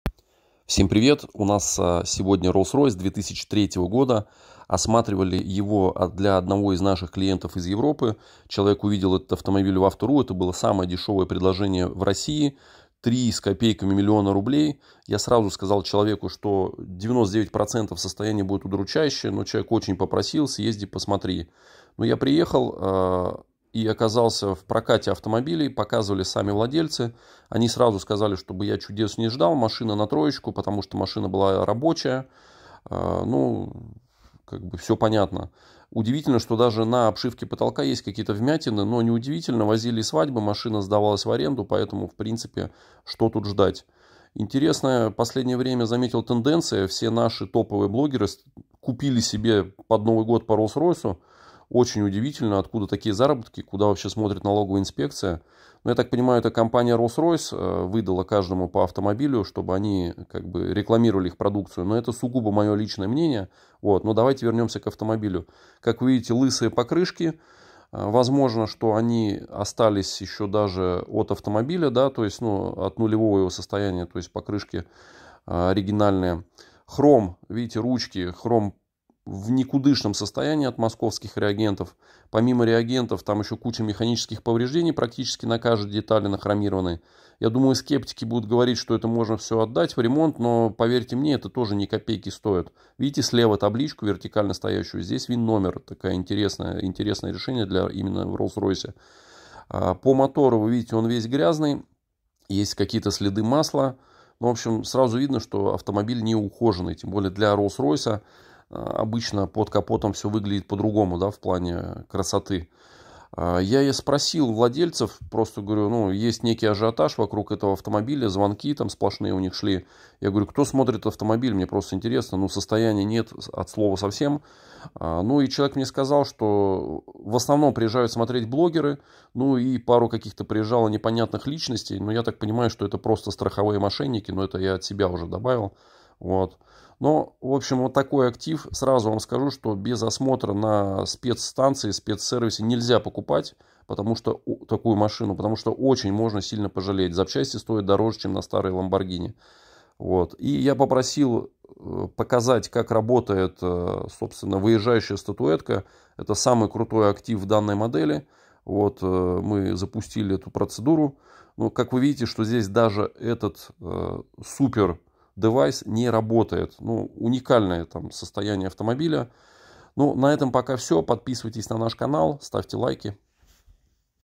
Роллс Ройс для блогера за 3 миллиона рублей!
Приехал смотреть самый дешевый Роллс Ройс в России!